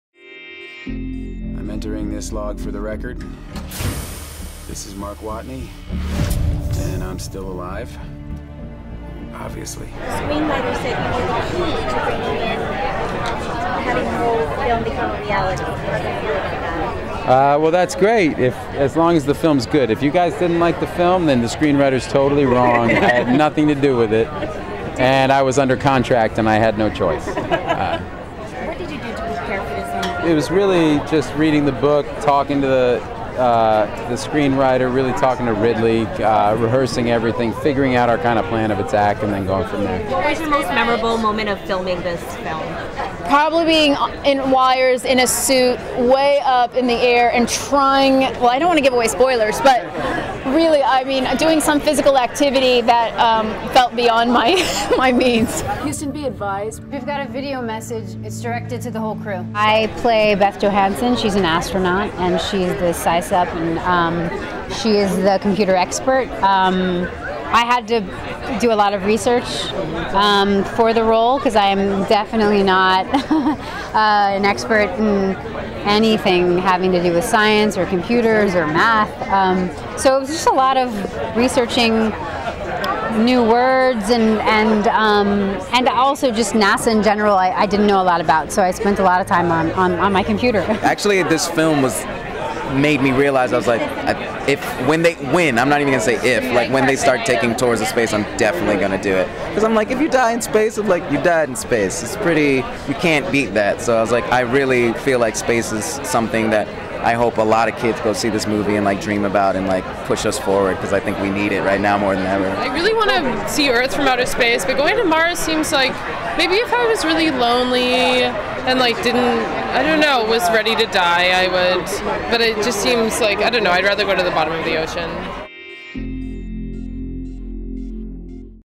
Matt Damon, Jessica Chastain walk the red carpet at TIFF
Matt Damon, Jessica Chastain, Kate Mara and Donald Glover were among the stars promoting "The Martian" at the Toronto International Film Festival Friday. Glover says working on the movie got him interested in visiting space. (Sept. 11)